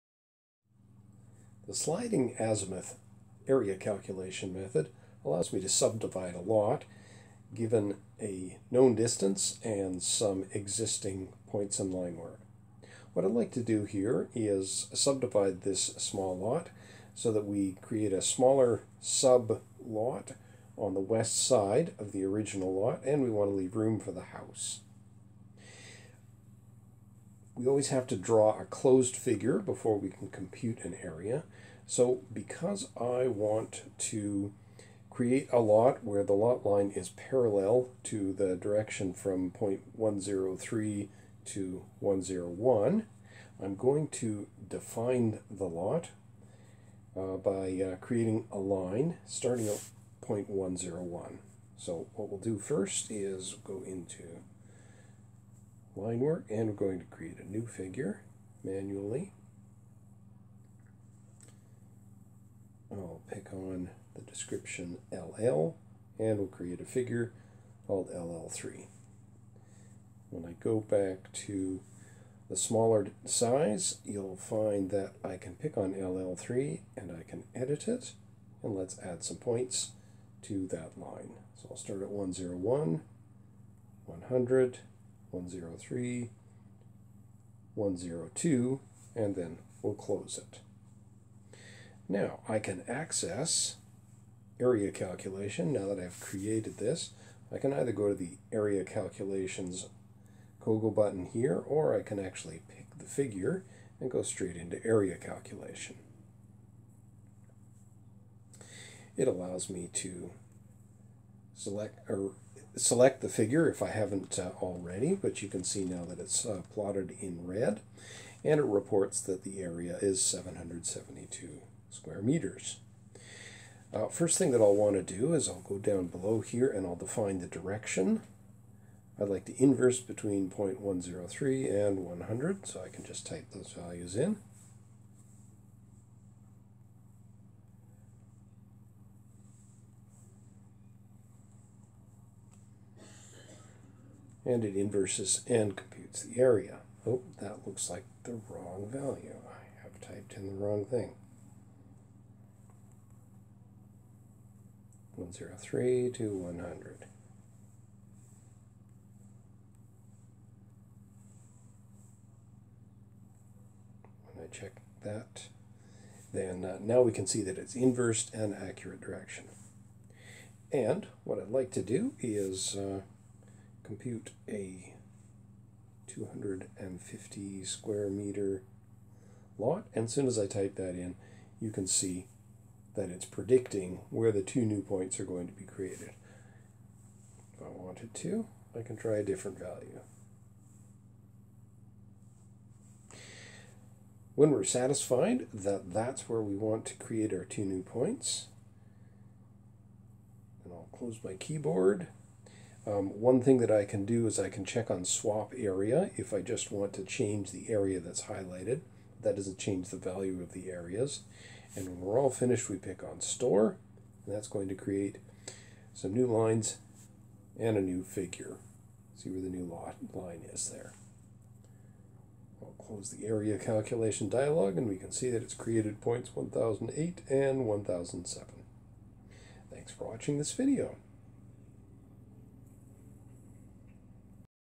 FieldGenius for Android 2.1 - Area Calculation
The user is now able to subdivide areas on closed figures to compute a new area.